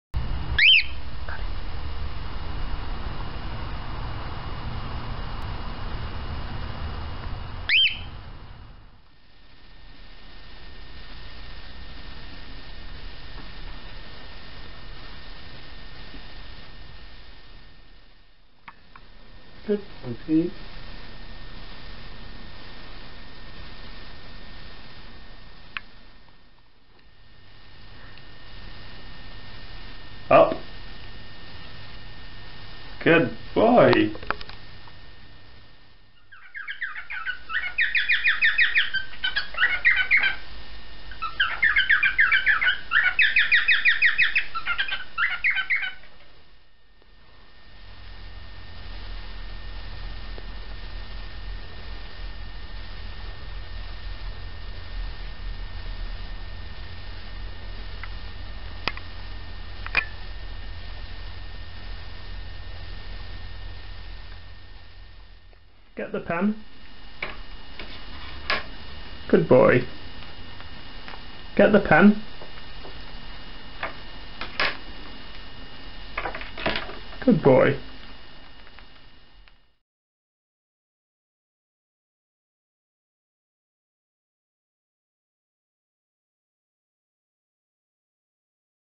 Early days with Pete the Cockatiel
It was 10 years ago today we brought Pete home, here's a few clips from his first few months with us.